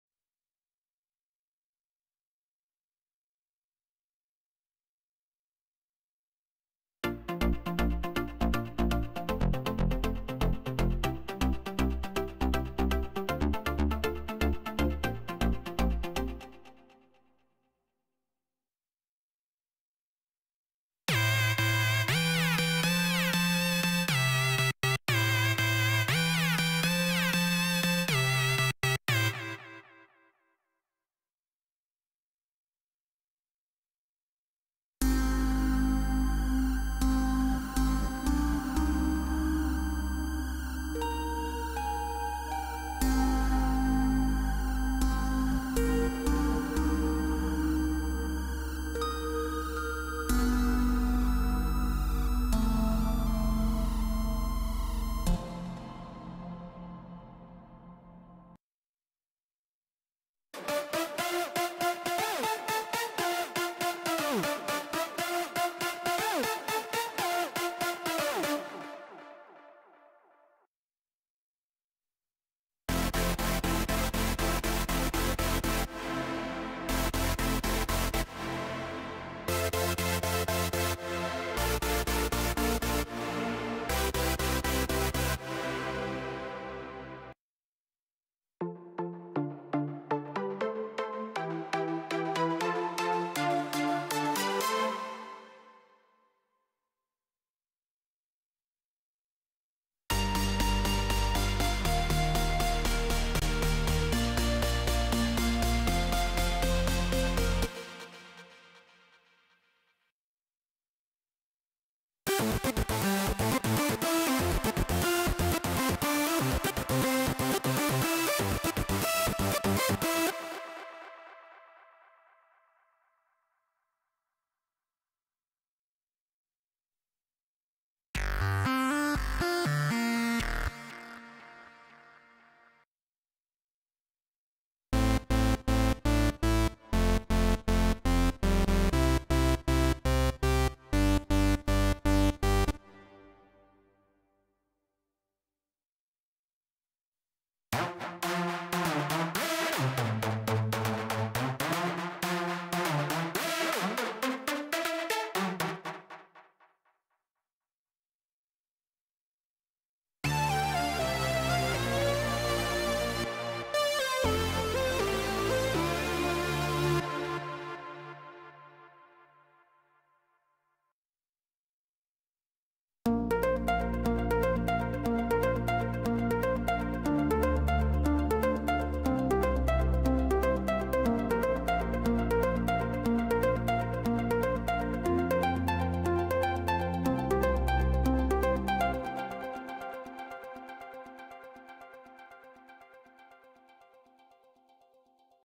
Rapid 1.8 | Melody Presets
RAPID is a modern polyphonic hybrid synthesizer designed to bring together fast workflow and powerful synthesis techniques. The unique layer engine that powers RAPID lets you shape and customize all aspects of your sounds, resulting in dynamic and fat patches, ready for production, performance or simply inspiration.